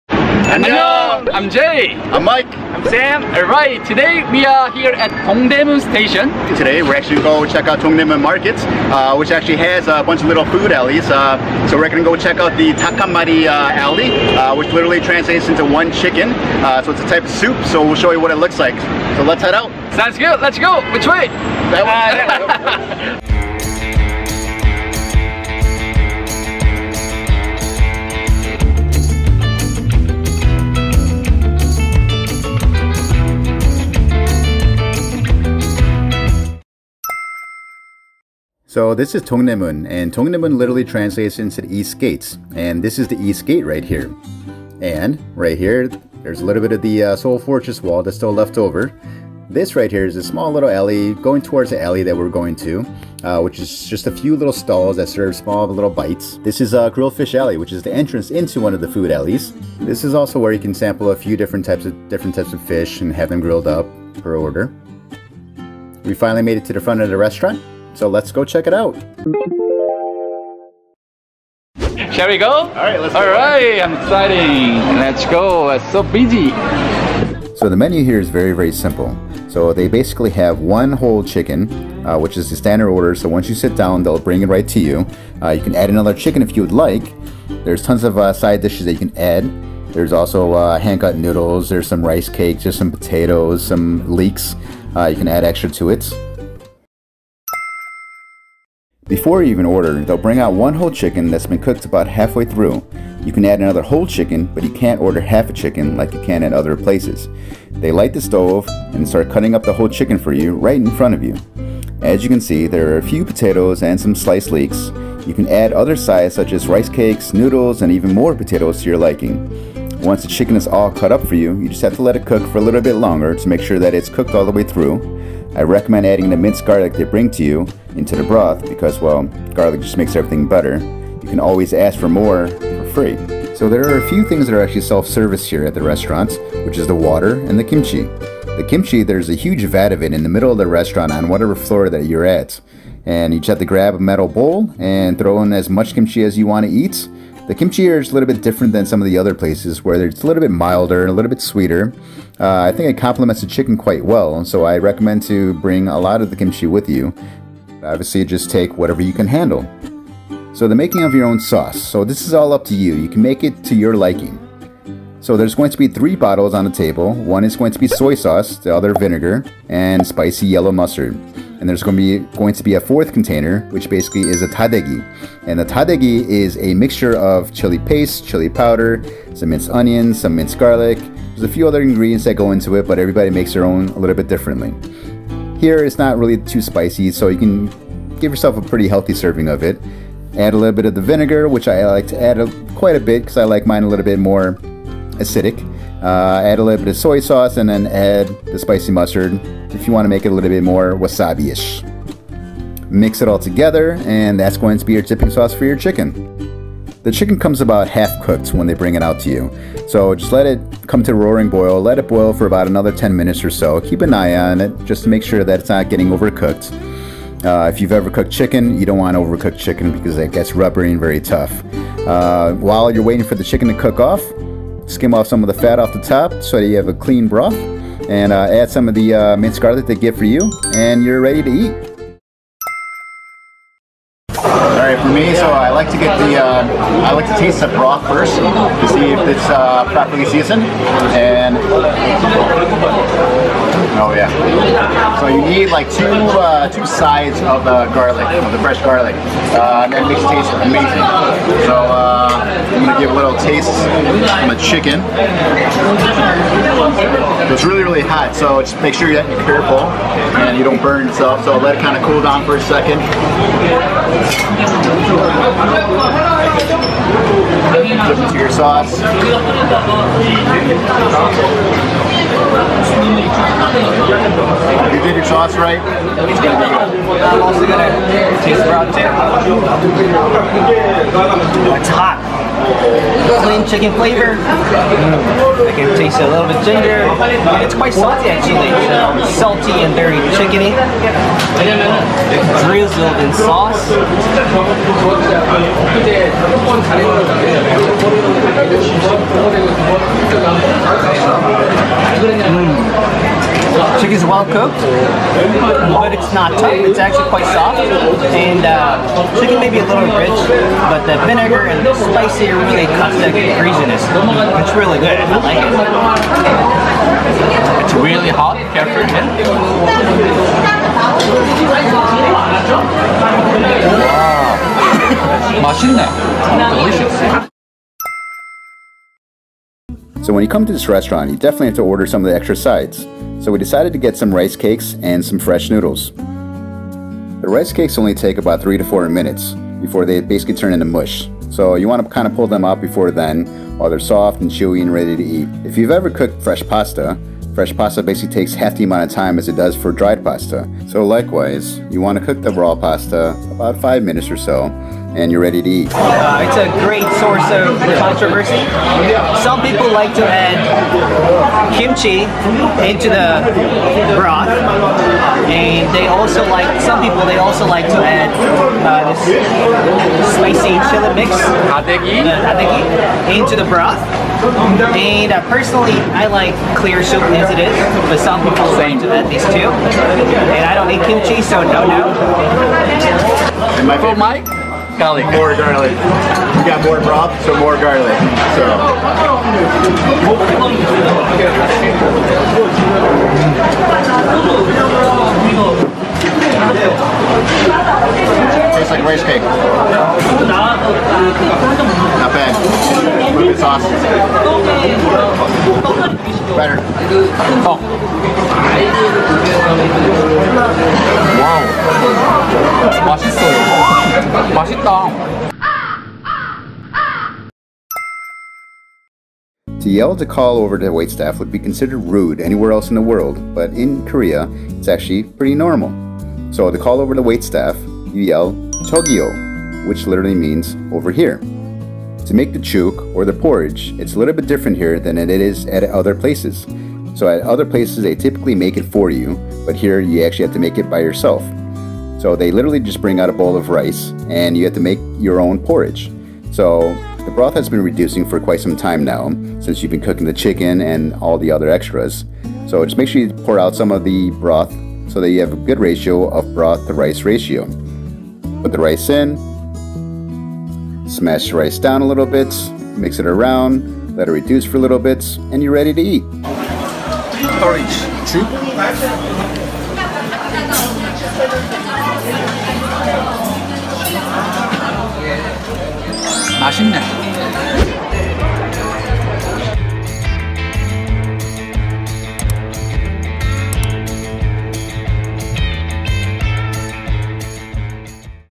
The Most Famous "One Chicken" Shop in One Chicken Alley
Baek-sook (백숙) : Boiled meat, usually refers to chicken.
It takes a while to prepare because it cooks for a long time.
It can be made with few ingredients such as a few aromatics and rice, but nowadays restaurants tend to use a lot of medicinal herbs and spices. Thus, making it expensive too.

Sam-gye-tang (삼계탕) : It translates to ginseng chicken soup, literally.
Korea's masochistic remedy to steamy hot summers.
It cannot be called Sam-gye-tang without ginseng.
While 'one chicken' (닭한마리) or Baek-sook (백숙) is a shared dish. Sam-gye-tang is served individually.

진옥화할매원조닭한마리 (Jinokhwa Halmae Original Chicken Hanmari)

A popular restaurant from the grilled fish alley (other restaurants also taste good)